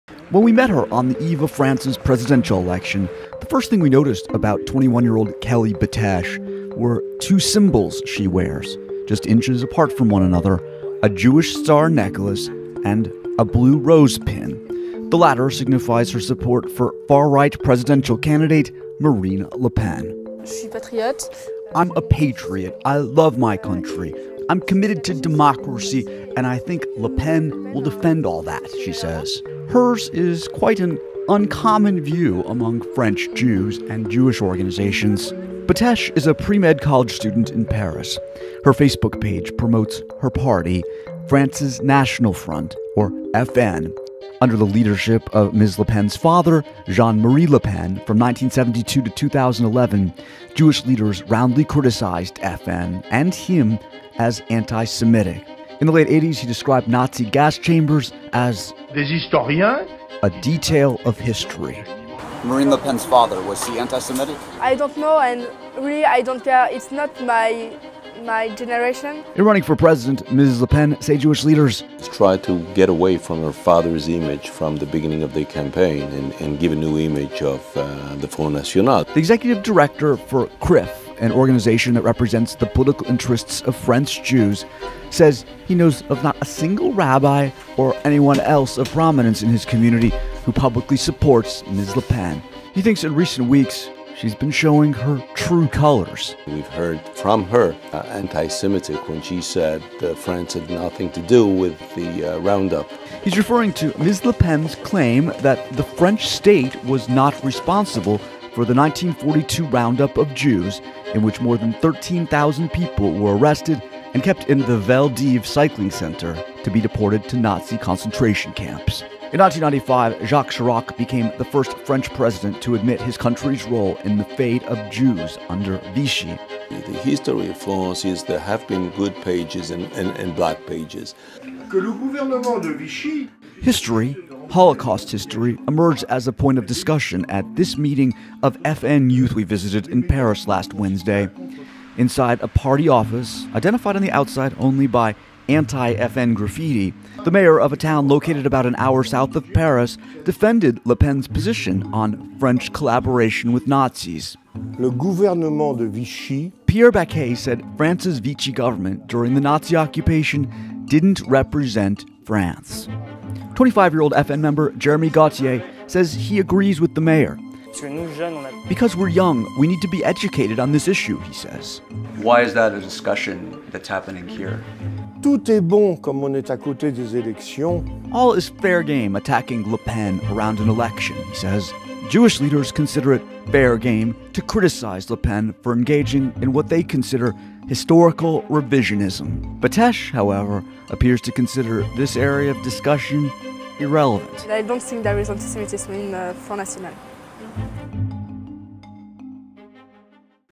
French Election: Young, Jewish Parisian Defends Le Pen
French presidential candidate Marine Le Pen’s efforts to "un-demonize" the National Front party has turned it into one of France’s largest political forces. Meet a young, Jewish Parisian who supports Ms. Le Pen, despite what French Jewish leaders are saying. WSJ's Jason Bellini reports. Photo: Matt McDonald.